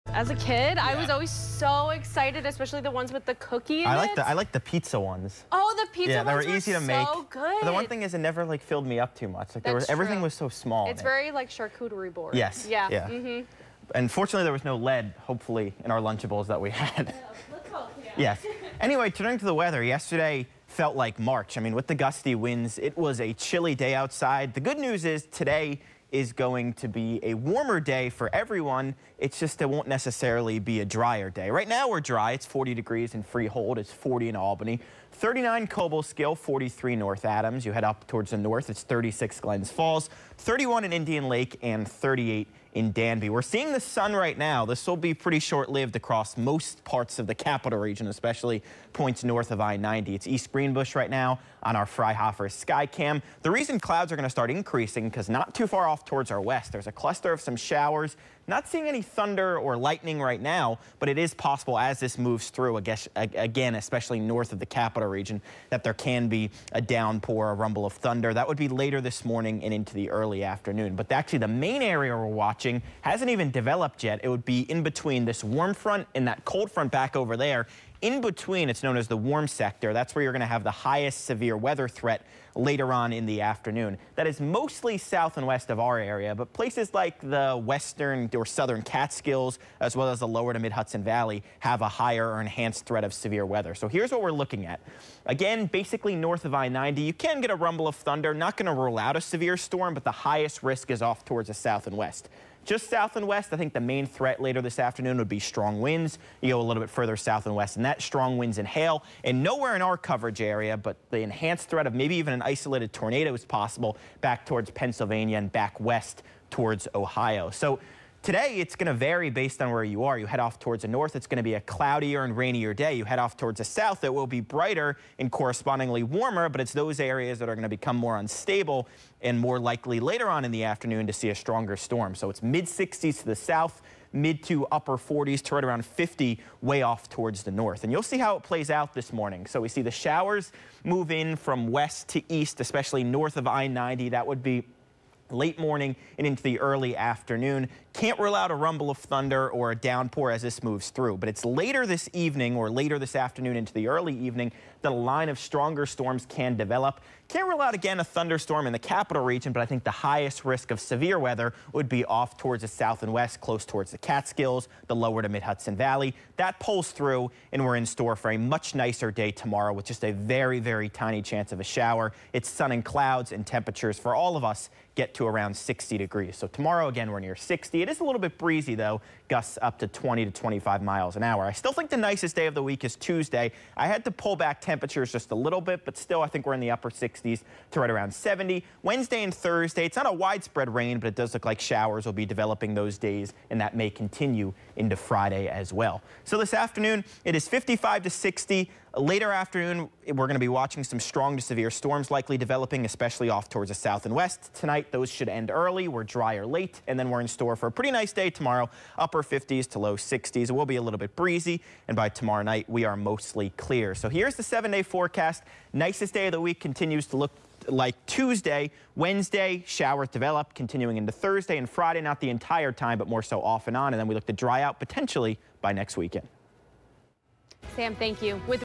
PM Severe Storms Mainly S&W Sam Coplin 4/14/24 Weather Forecast
After some morning and early afternoon showers, a line of stronger storms looks to develop this afternoon and early evening esp. for points S&W. Nicer days lie ahead to start the work week.